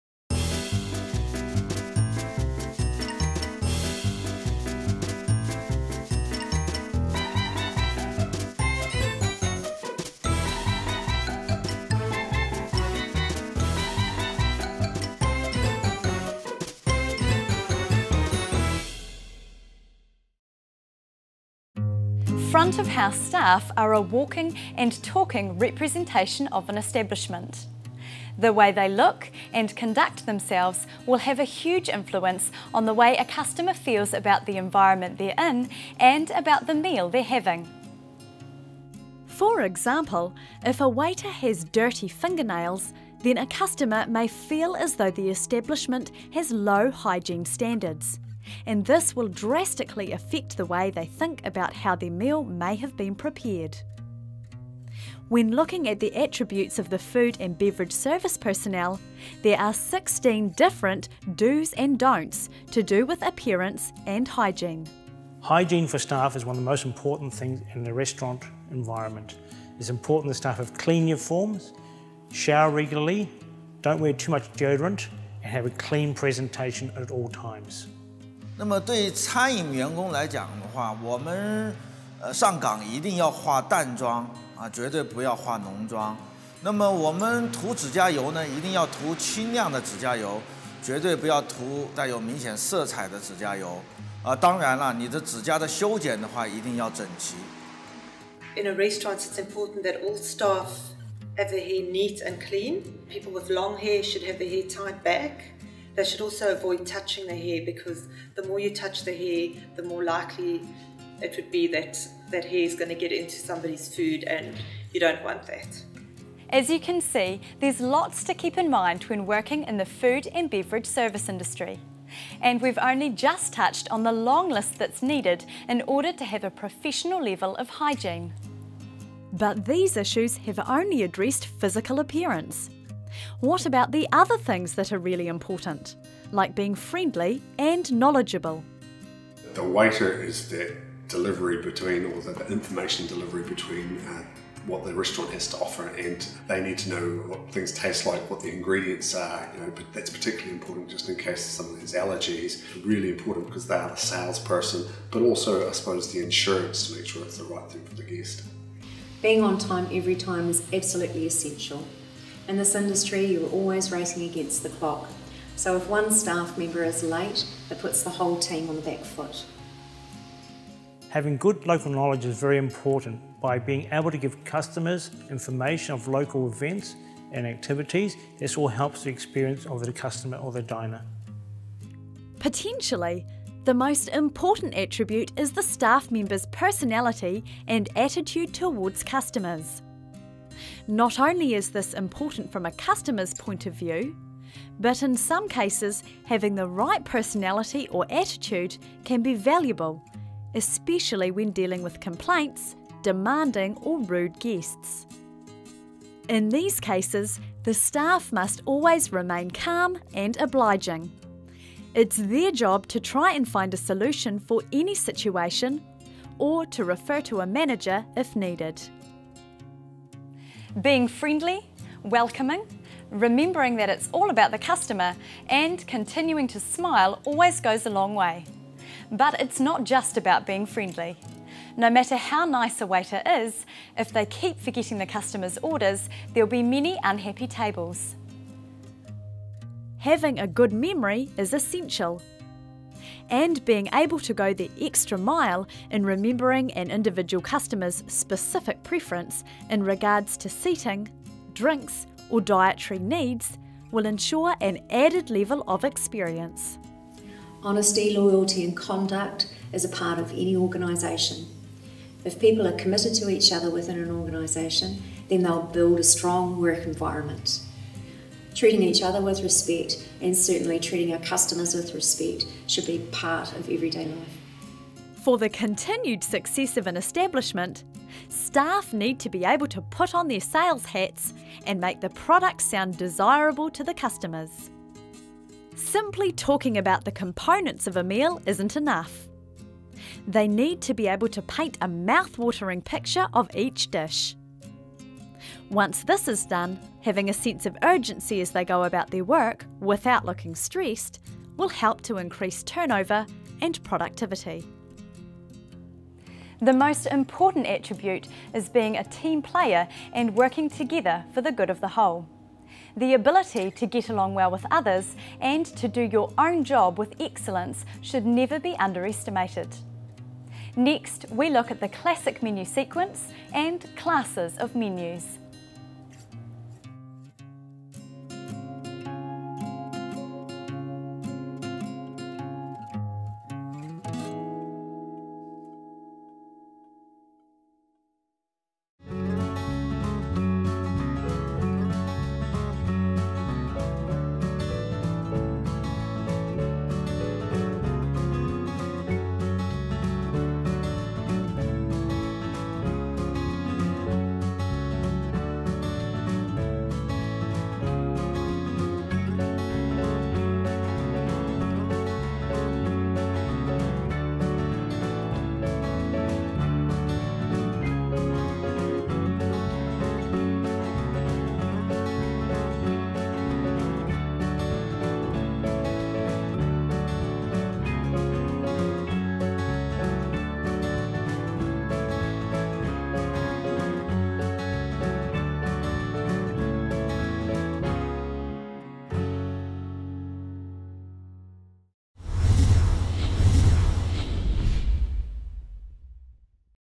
Southern Institute of Technology - Dip. Hotel & Tourism: Series 3 Episode 6
- Explore various areas in the world of hospitality in this 12 episode series looking at Hotel & Tourism Management.